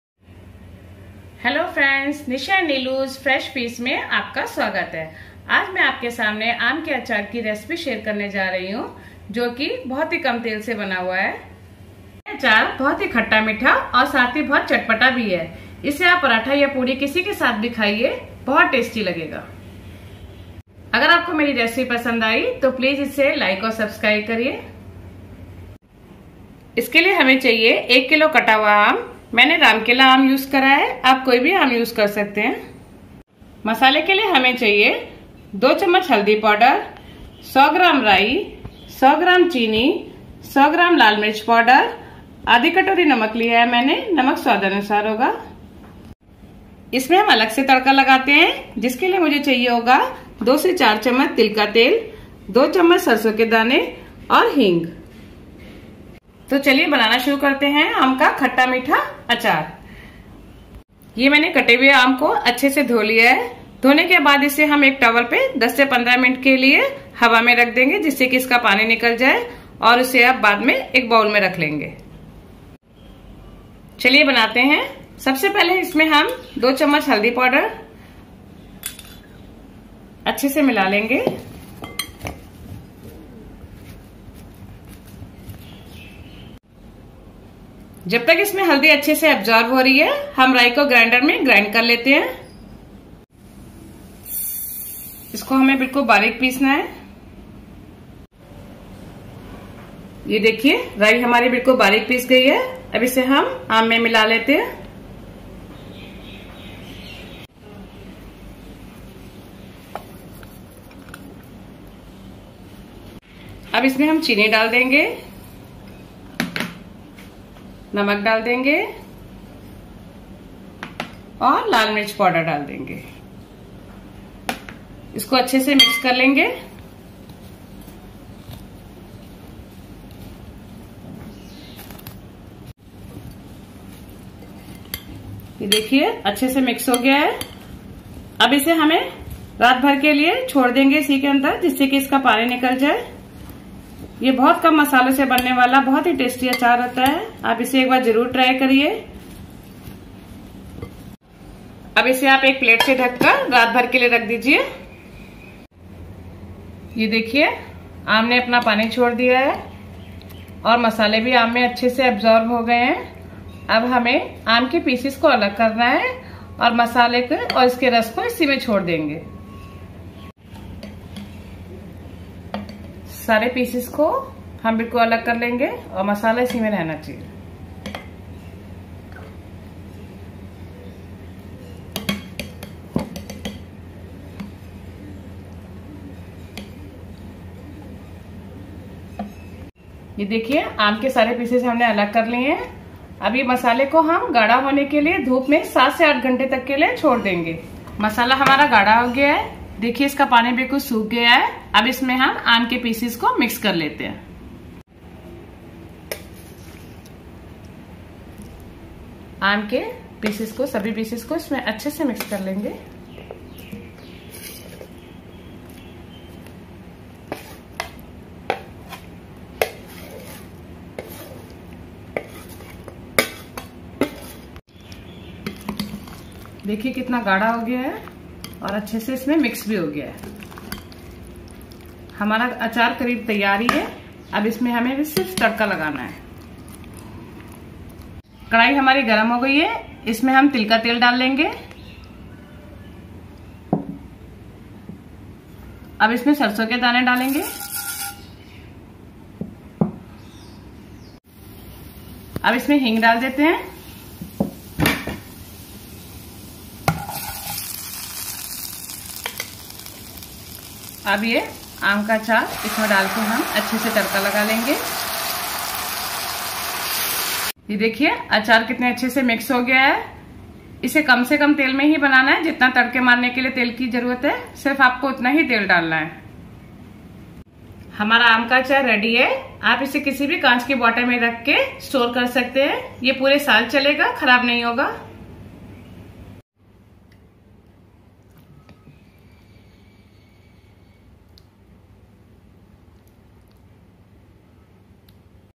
Khatta Meetha Chatpata Aam Ka Achaar | Oil Free Aam Ka Achaar | South Indian Mango Pickle
Raw mango pickle recipe, khatta Meetha Chatpata with out oil Mango pickle. This mango pickle is made with only 3 ingredients. You can store it for 1and 2 years.